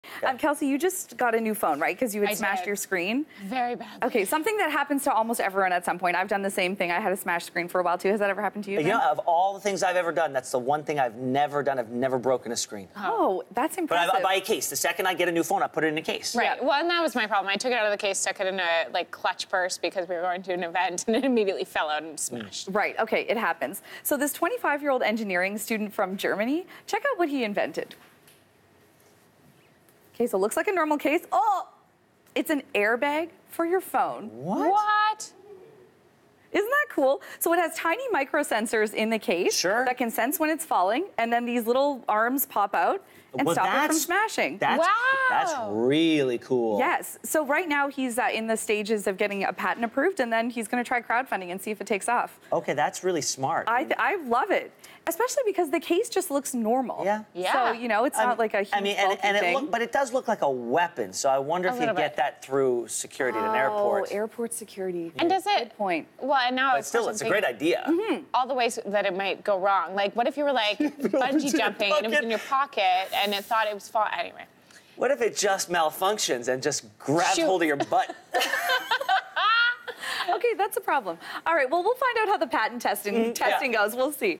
This is the phone case we’ve all been waiting for | Your Morning
Philip Frenzel has created an “airbag” phone case that senses when your phone is in free fall and releases prongs to protect it from damage.